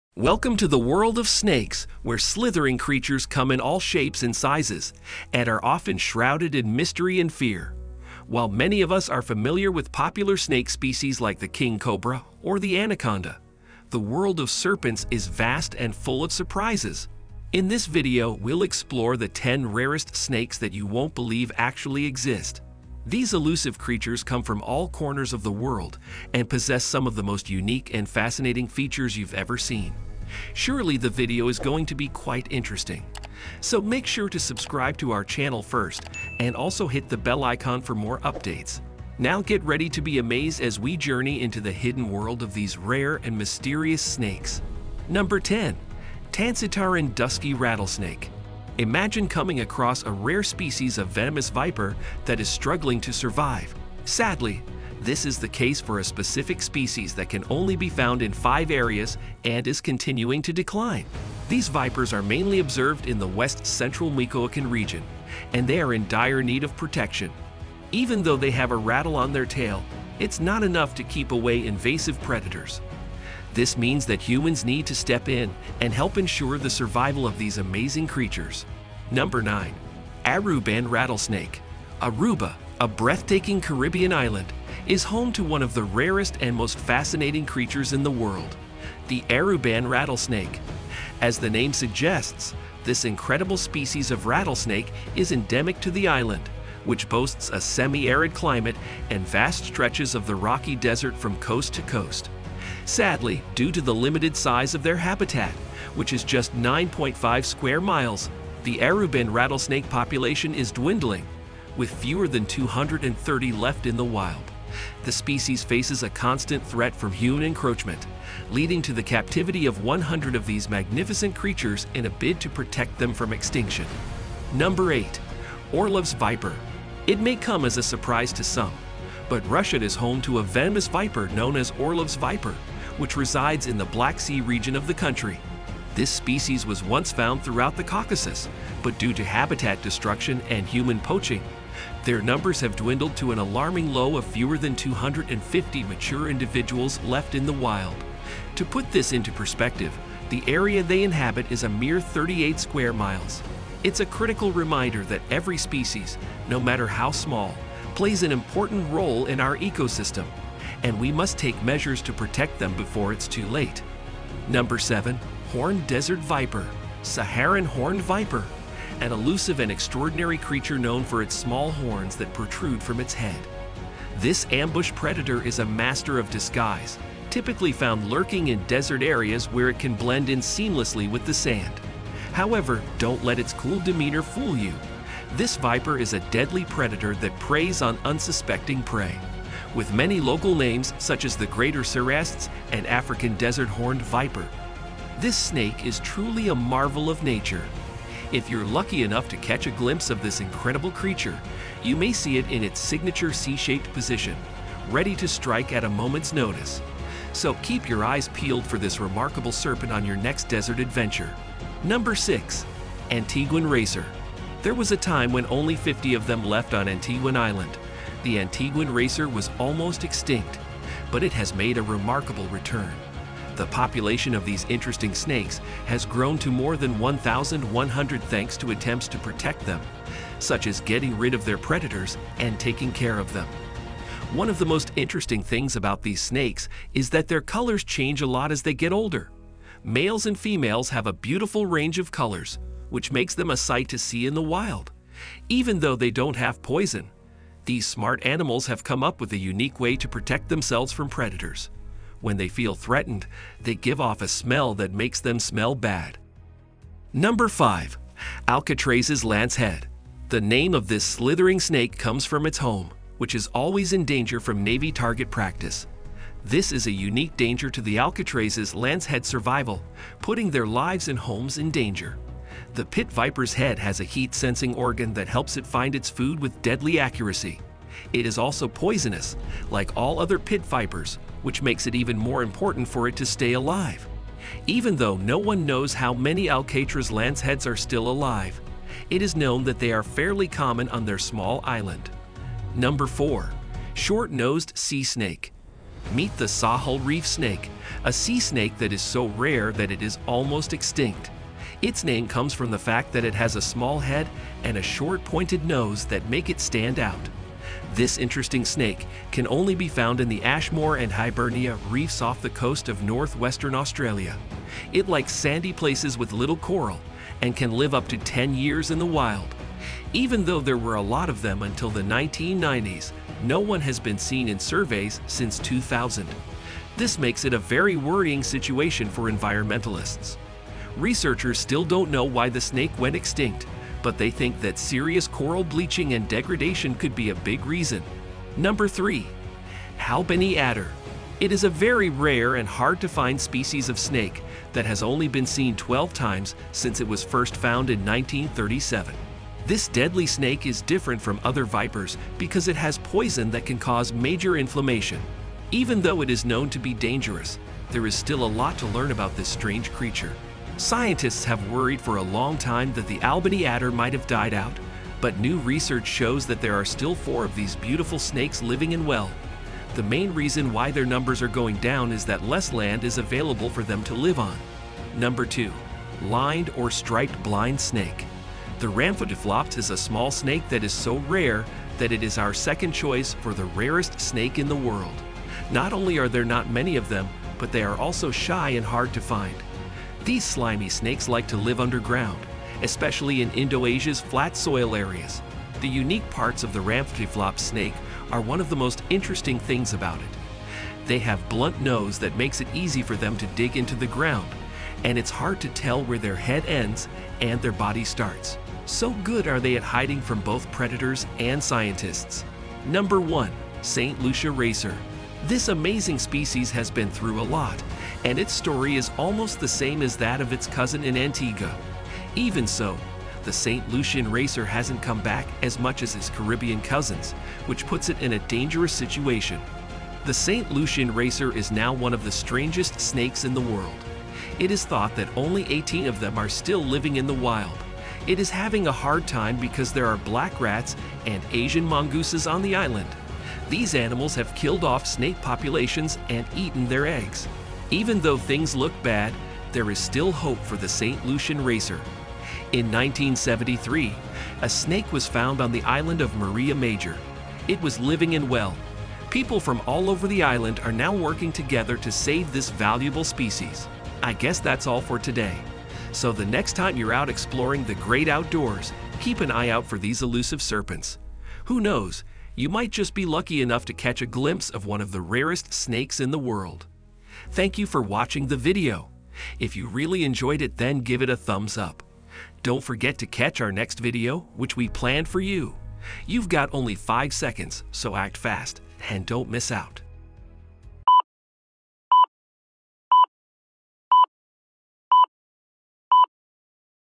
15 Rarest Snakes In The World
"15 Rarest Snakes In The World" In this video you will see 10 fascinating, dangerous but also very rare snakes.
Unfortunately many of these magnificent animals are left with very few specimens alive. Maybe when I would watch this video some of them could be extinct, so let's remember to respect the world we live in to allow all species however poisonous or dangerous to continue to reproduce.